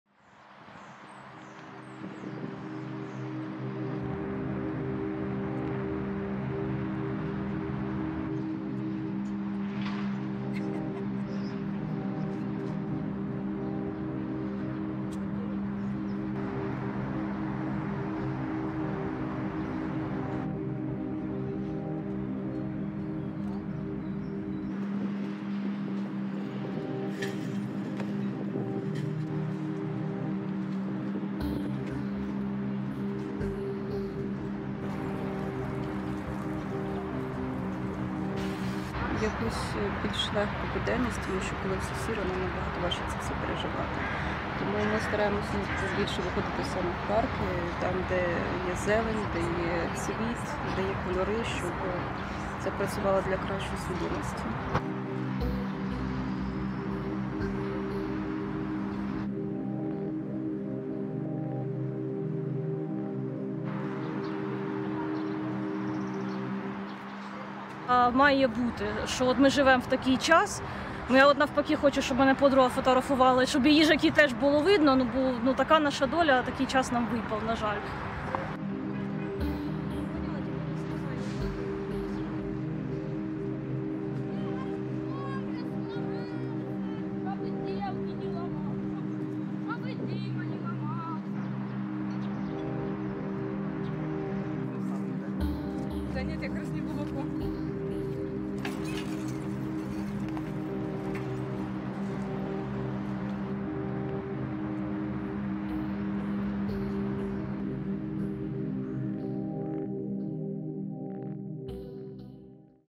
GUERRA UCRANIA: La PRIMAVERA llega a la ciudad de KIEV, un ALICIENTE para muchos CIUDADANOS | RTVE
Un año después de que que los ucranianos recuperaran el control de la zona en abril del año pasado, el parque Kyoto, de ambientación japonesa, situado en la capital ucraniana, ofrece estos días el contraste del rosa de los cerezos en flor con el gris de las barreras antitanque y los alambres de espino.

los ciudadanos de Kiev disfrutan de un paseo mientras saltan diferentes obstáculos creados por la guerra. De entre los restos de las trincheras, campos de minas y bunkers la naturaleza pervive y se muestra tan bella como lo ha hecho siempre. Los visitantes no han dejado pasar la oportunidad de admirar el pequeño bosque nipón e inmortalizar el momento.